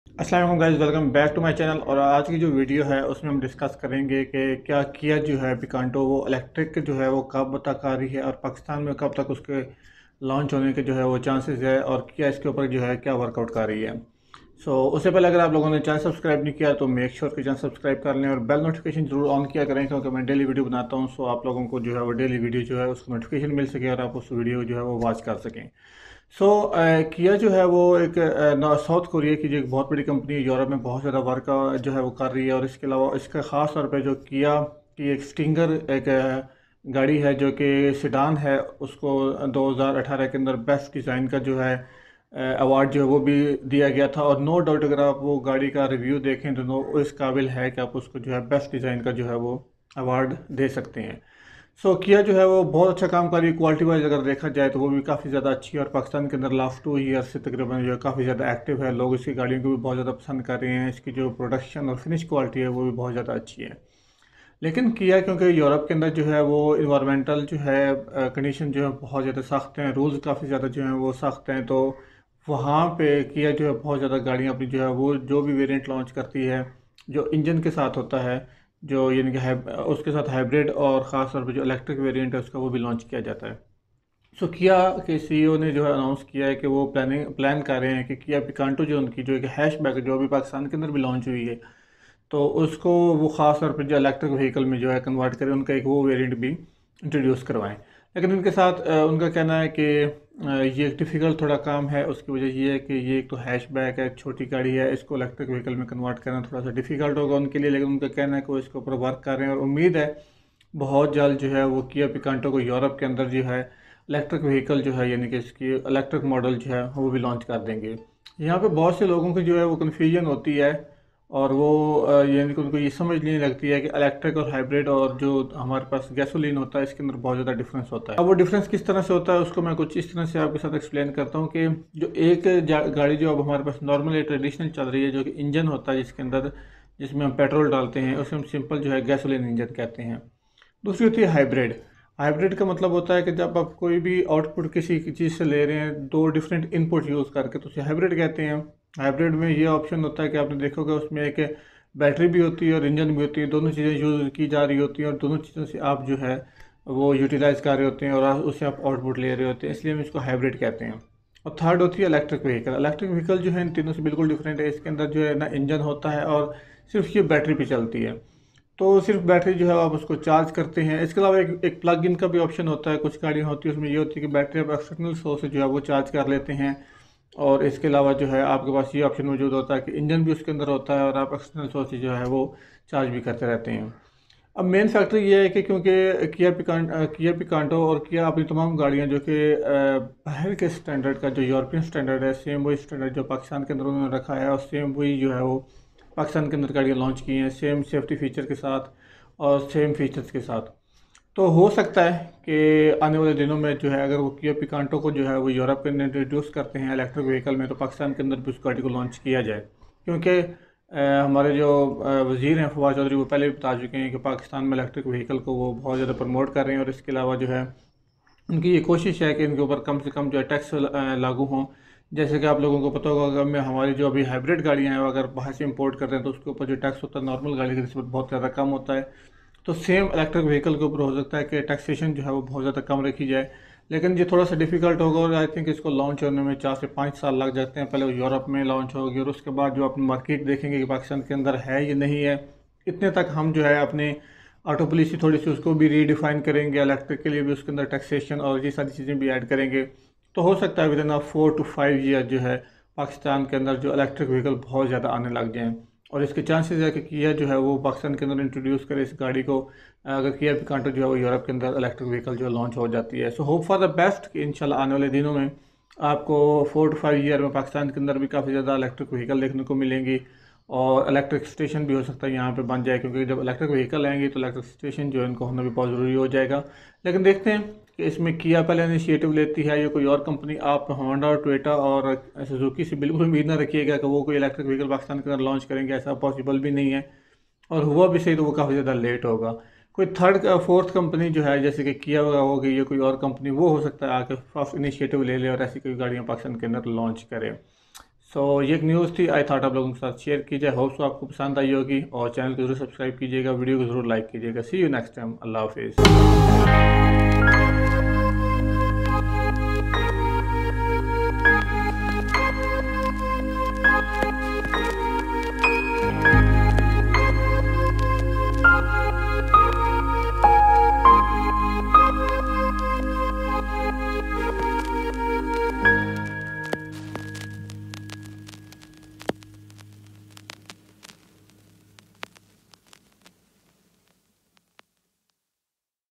When Electric KIA Picanto is going to Launch in Pakistan by AutoWheels
KIA is thinking to launch Electric KIA Picanto in Globally and may be they also launch Electric KIA Picanto in Pakistan as well so hope for the best.